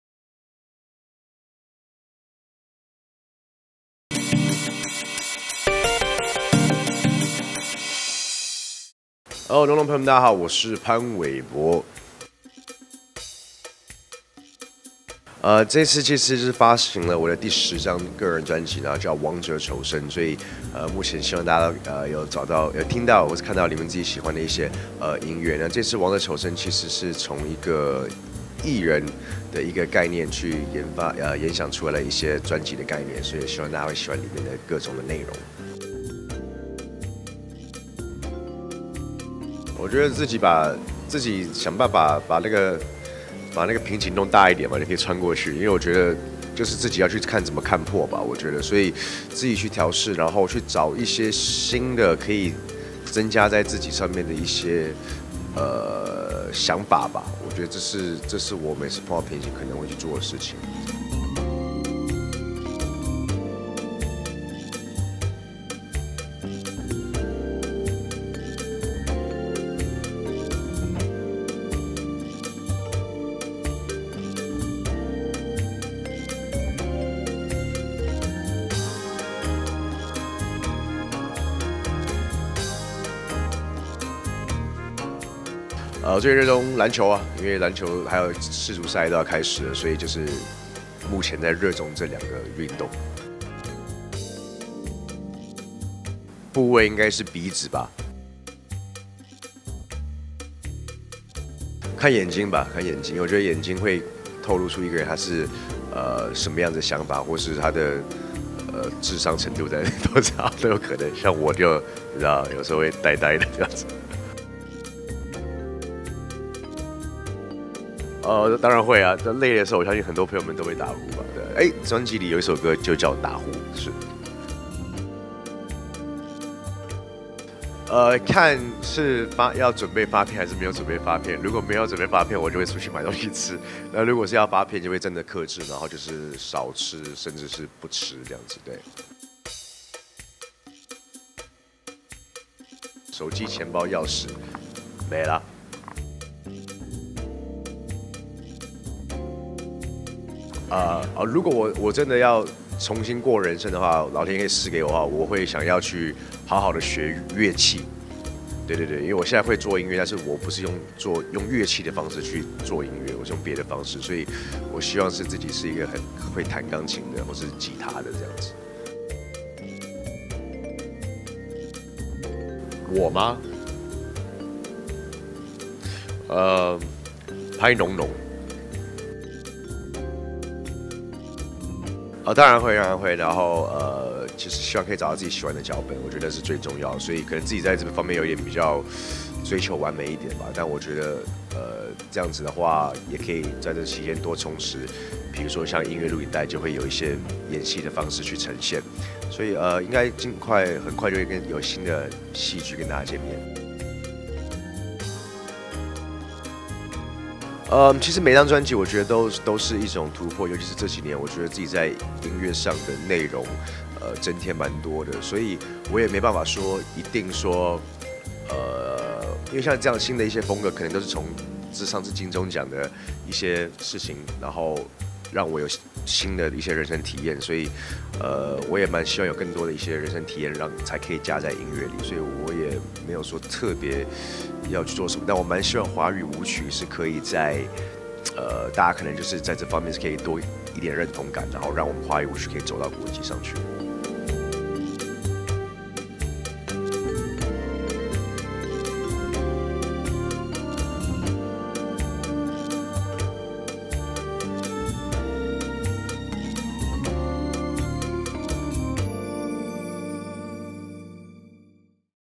《bella儂儂》2014年7月號 bella men 潘瑋柏
VJ、歌手、影帝、創業家. 潘瑋柏身上，堆疊著多重角色，每個都演得絲絲入扣，他歸咎於這是小丑本分，實際上，他是游刃有餘的天生玩咖。